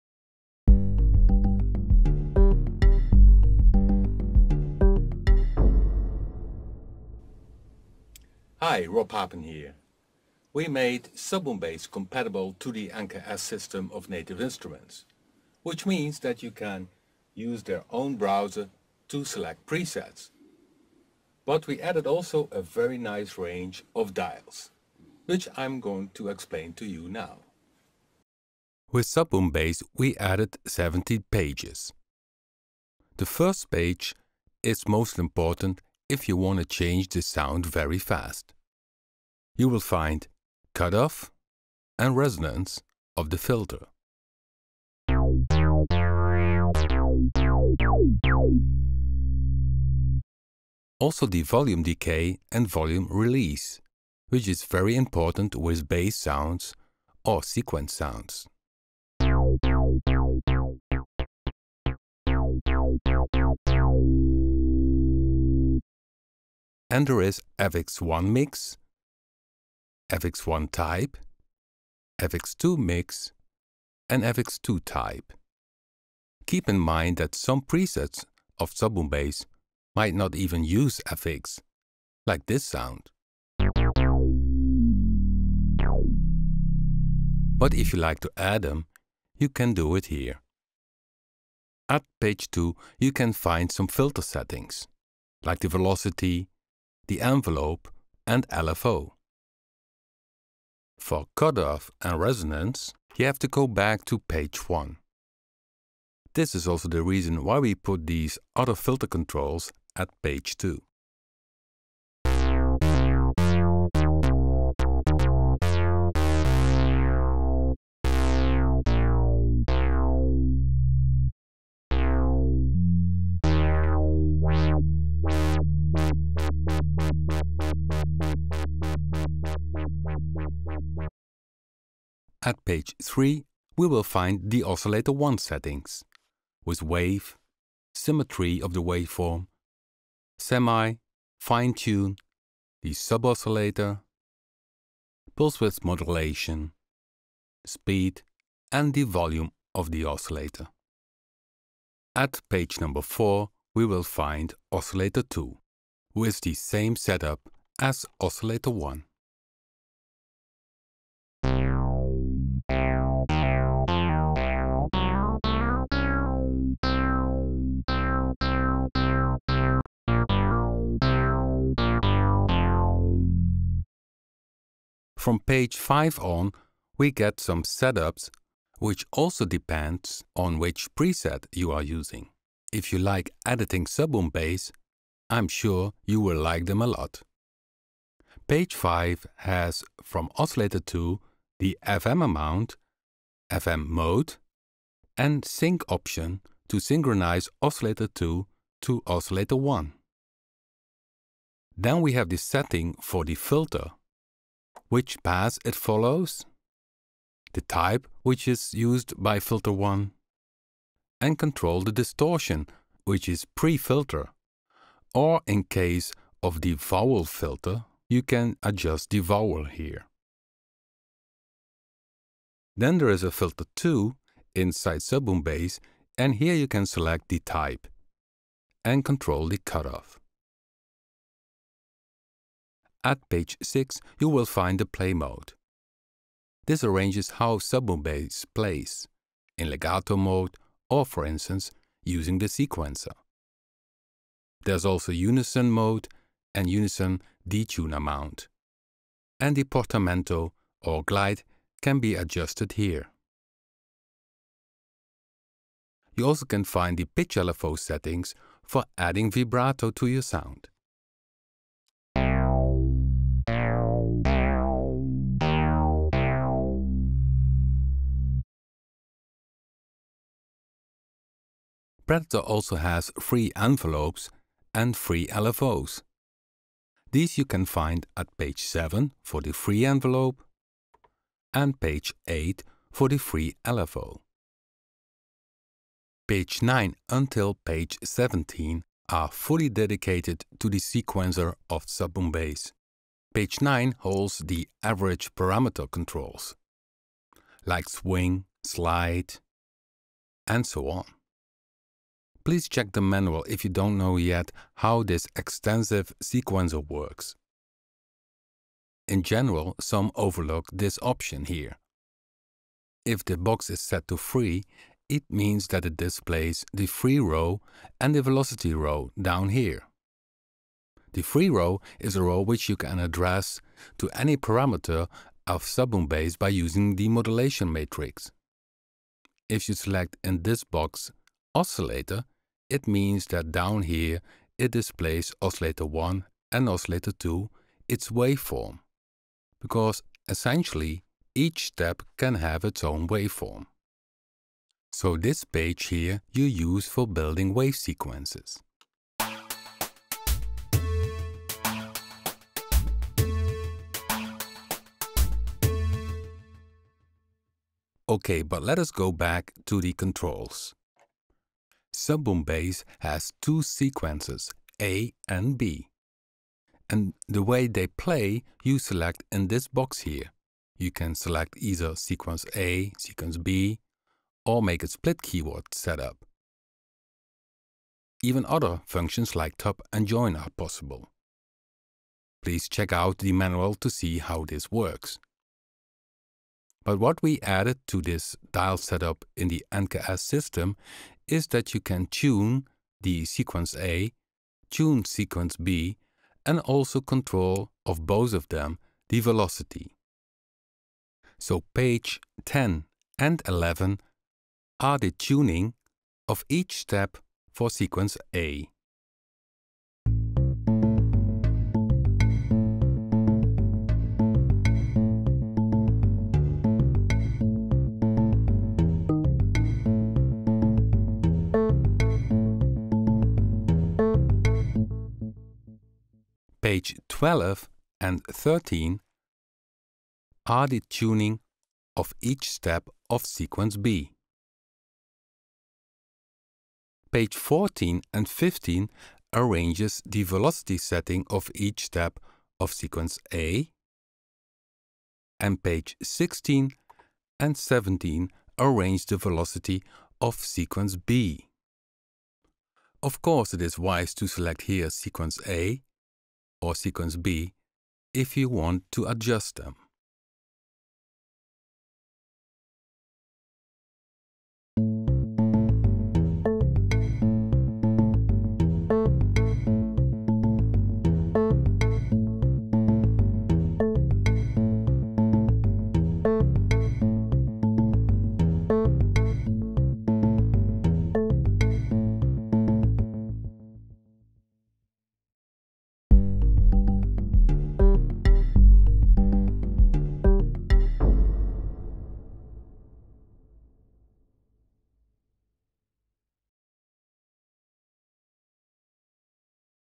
Rob Papen SubBoomBass NKS
SubBoomBass is compatible with the NI NKS system: Machine and Komplete Kontrol. This video explains the layout of the dials/controllers for SubBoomBass